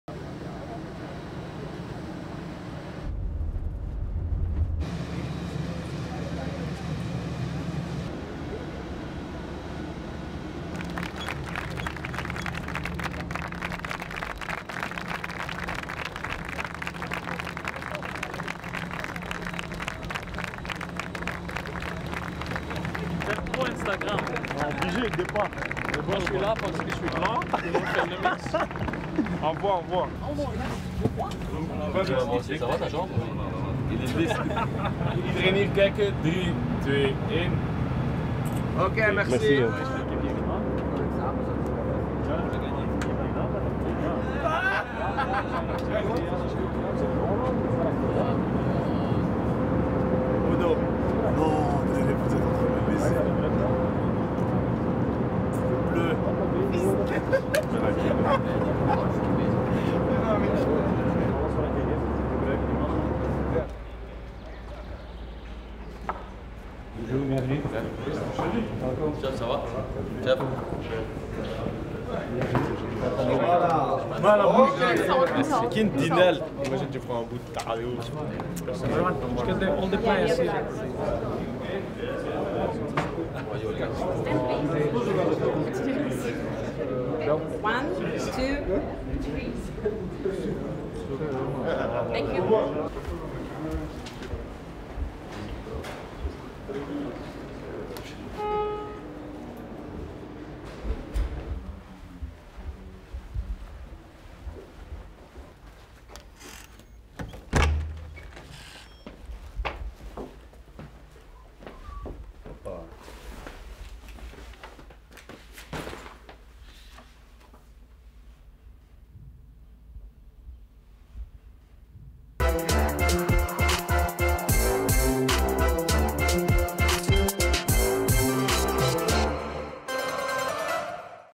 Russia | Travel day to Russia
13/06/2018 - Moscow (Russia). Travel day to Russia! ✈🇷🇺